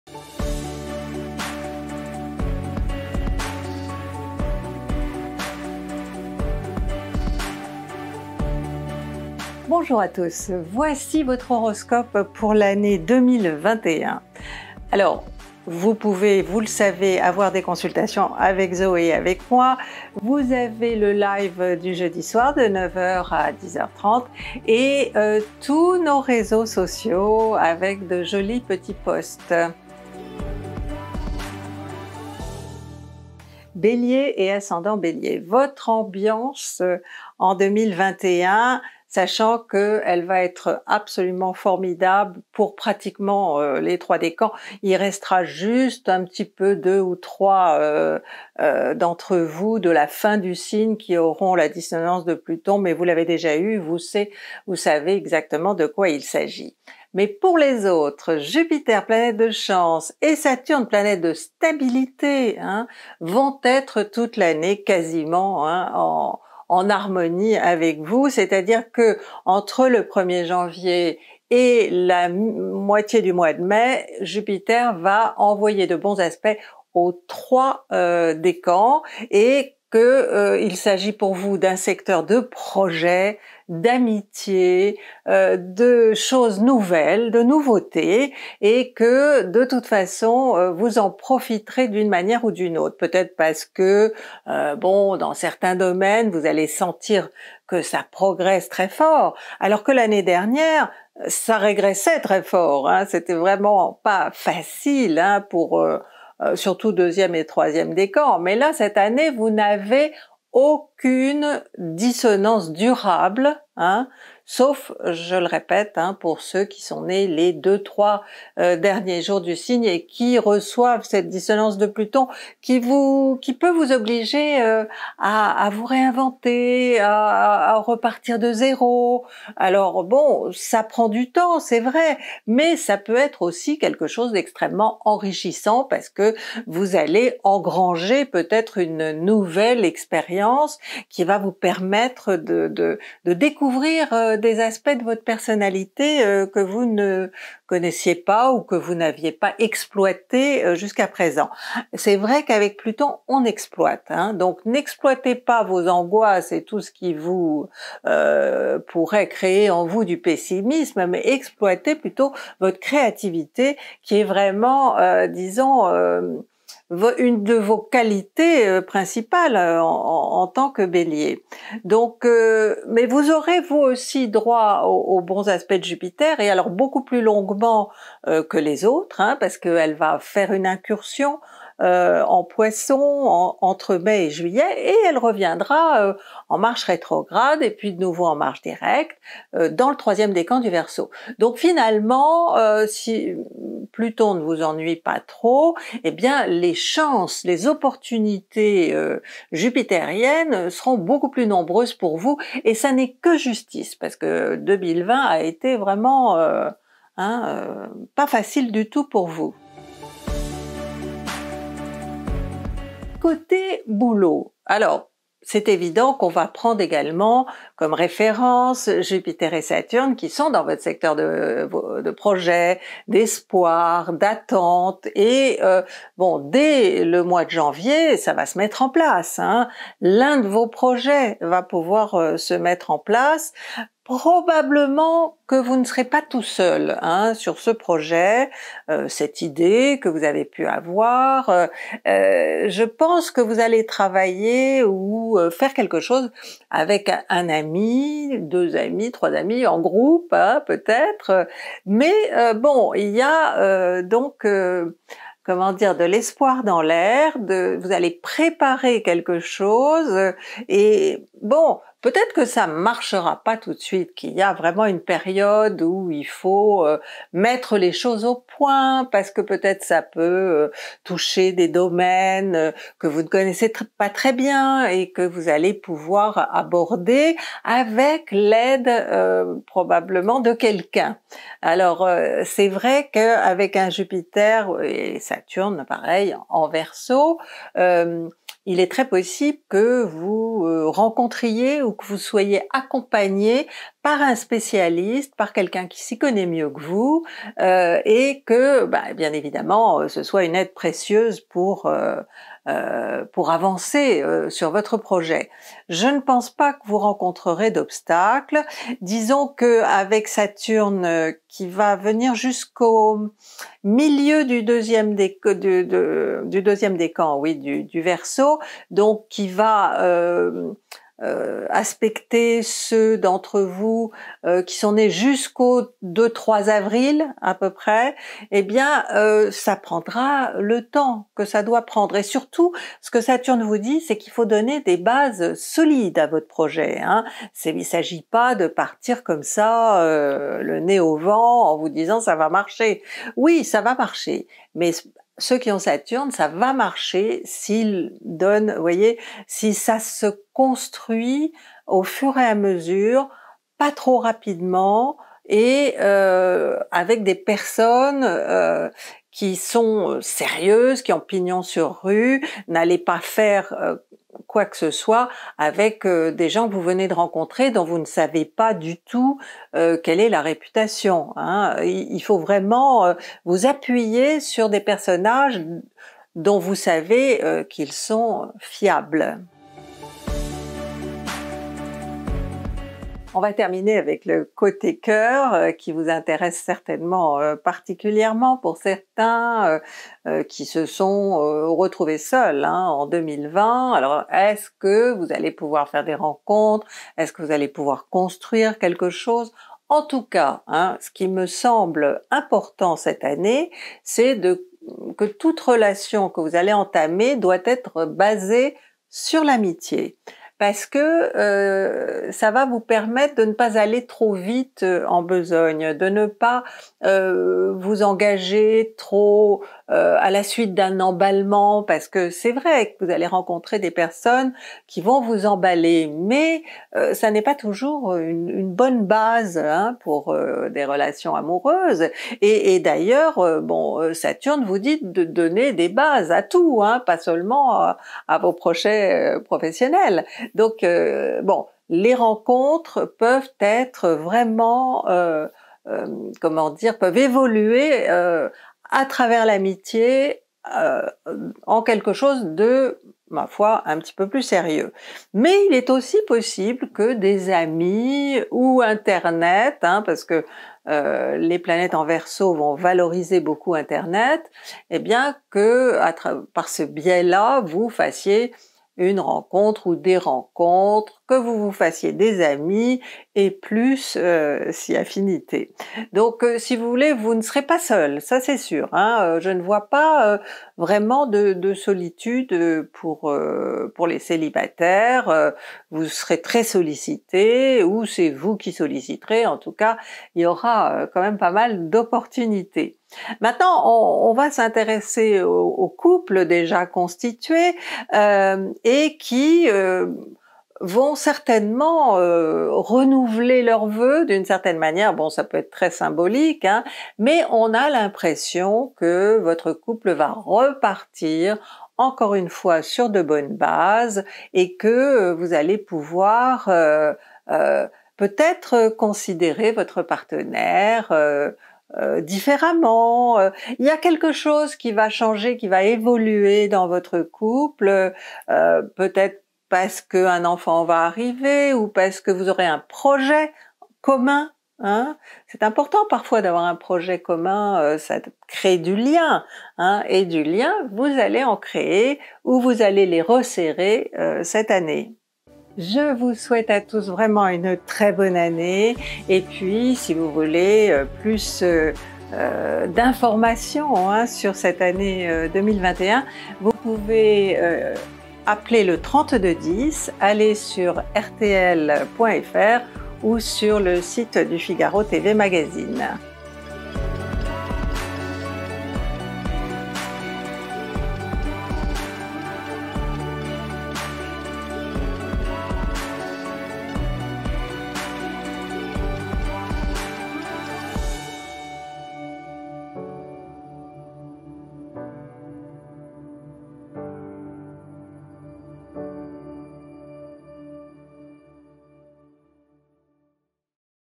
Horoscope Bélier ♈️ 2021: une année de progrès 📈
🌟 horoscope du Bélier pour 2021, avec une partie ambiance, une partie travail/quotidien et une partie amour.
🌟 Pour tout savoir de votre année, regardez l'horoscope de votre signe astrologique et celui de votre Ascendant pour plus de précision.